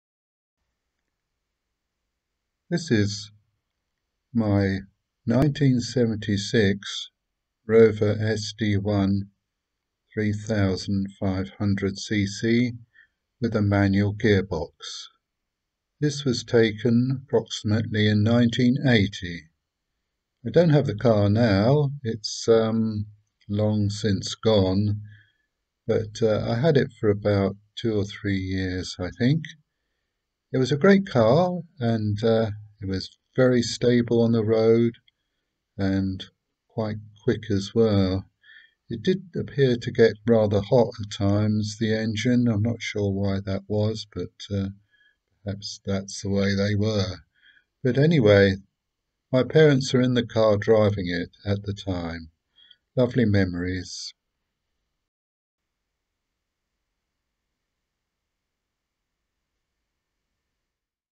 Rover 3500 SD1 taken 1980
My parents driving my Rover 3500 SD1 manual  filmed on super 8 movie silent🙂